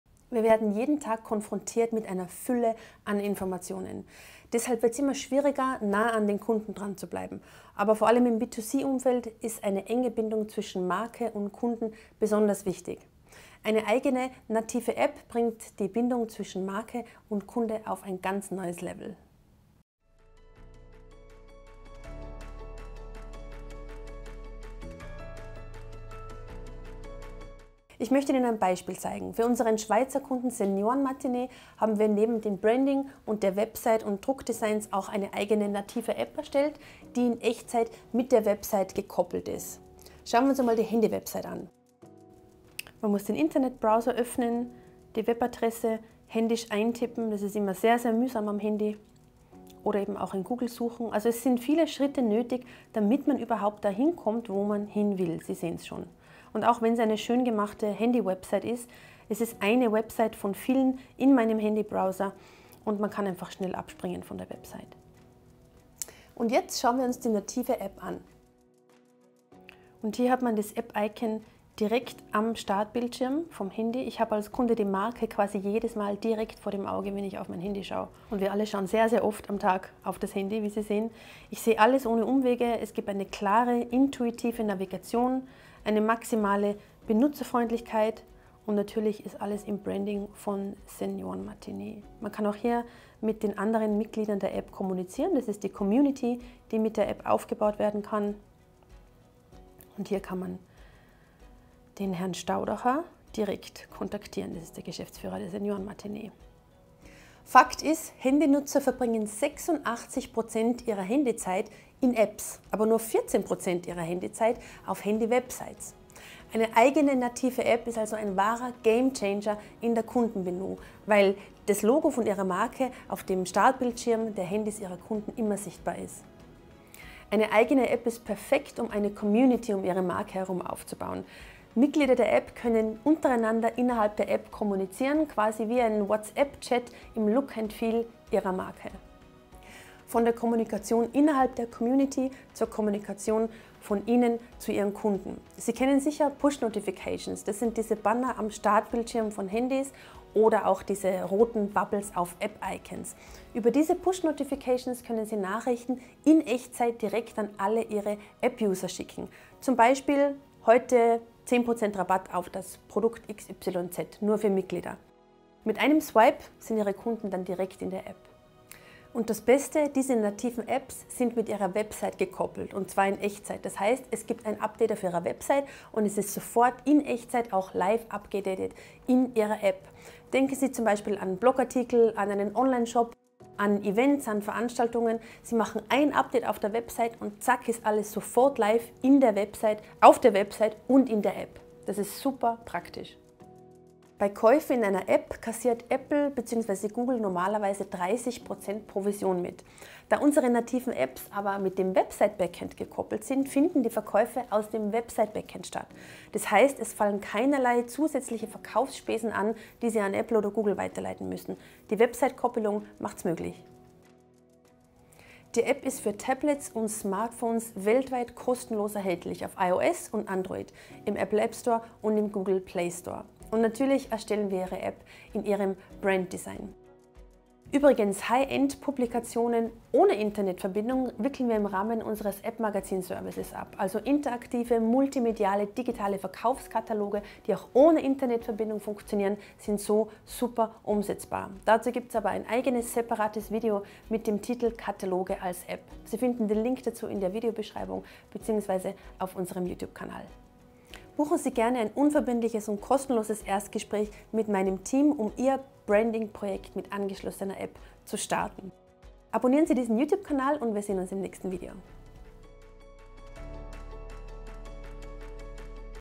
Stärkere Kundenbindung dank eigener App
Wir werden jeden Tag konfrontiert mit einer Fülle an Informationen. Deshalb wird es immer schwieriger nah an den Kunden dranzubleiben. Dabei ist ein enger Kontakt zwischen Marke und Kunden gerade im B2C-Bereich enorm wichtig. Eine eigene, native App bringt die Bindung zwischen Ihrer Marke und Ihren Kunden auf ein ganz neues Level.

In diesem Video erfahren Sie, wie wir das bei HCG corporate designs für unsere Kunden umsetzen und welche Vorteile dies bietet.

Die Produkte aus der HCG office collection, die Sie in diesem Video sehen, können hier erworben werden